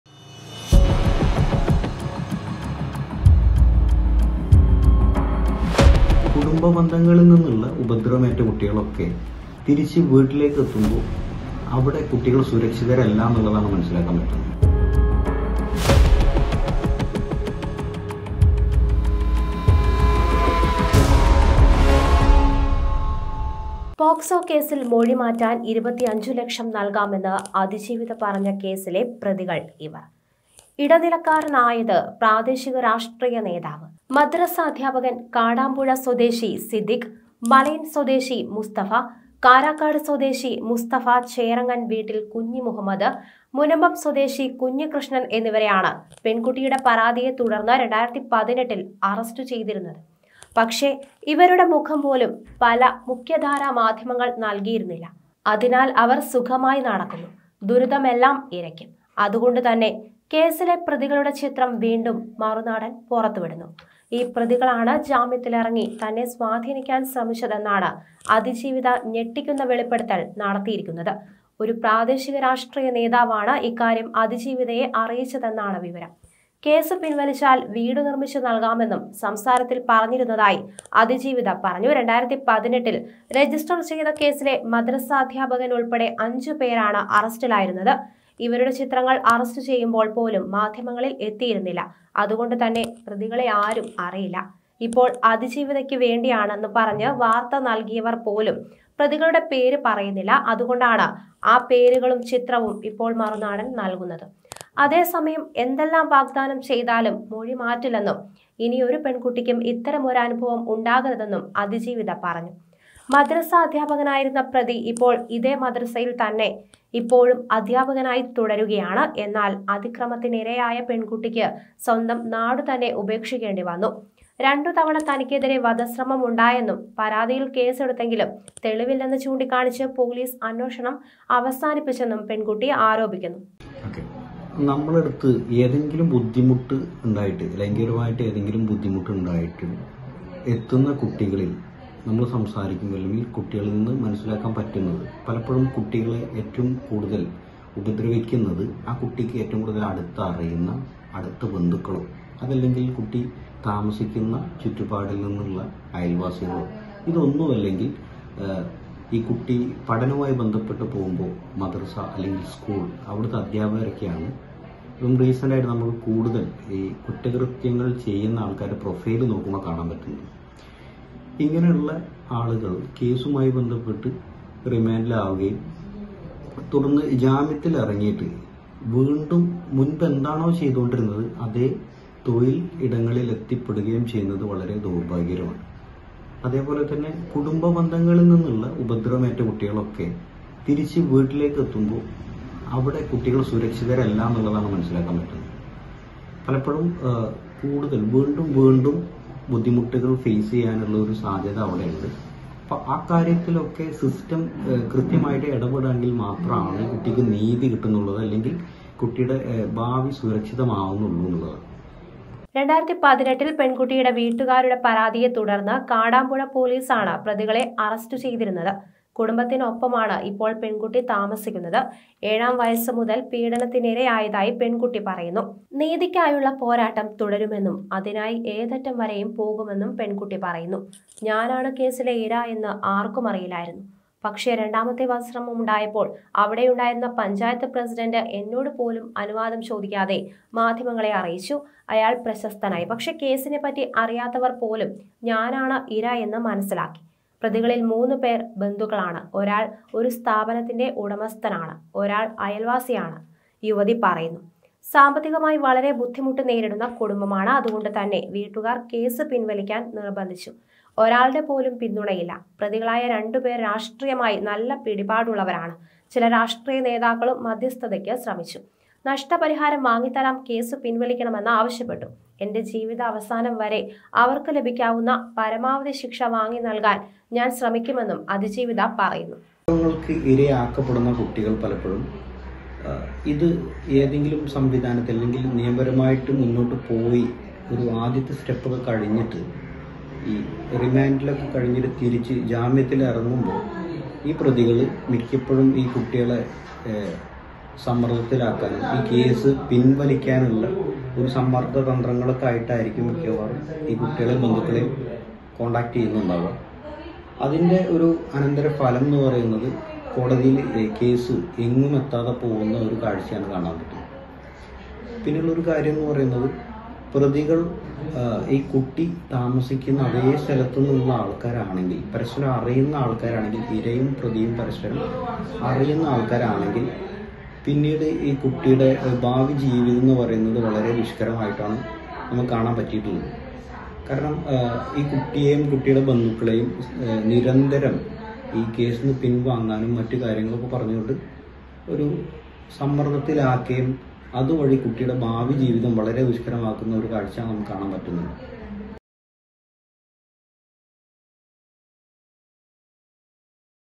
ആ ക്രൂരന്മാരുടെ ചിത്രം മറുനാടന്‍ പുറത്തു വിടുന്നു l malappuram
ഞാനാണ് കേസിലെ ഇര എന്ന് ആർക്കുമറിയില്ലായിരുന്നു; രണ്ടാമത്തെ വധശ്രമമുണ്ടായപ്പോൾ പഞ്ചായത്ത് പ്രസിഡന്റ് അനുവാദം ചോദിക്കാതെ മാധ്യമങ്ങളെ അറിയിച്ചു; അയാൾ പ്രശസ്തനായി; പക്ഷേ കേസിനെ പറ്റി അറിയാത്തവർ പോലും ഞാനാണ് ഇര എന്ന് മനസ്സിലാക്കി; മൊഴി മാറ്റാൻ വാഗ്ദാനം ചെയ്തത് 25ലക്ഷം; പോരാട്ടം തുടർന്ന് അതിജീവിത; ആ ക്രൂരന്മാരുടെ ചിത്രം മറുനാടൻ പുറത്തു വിടുന്നു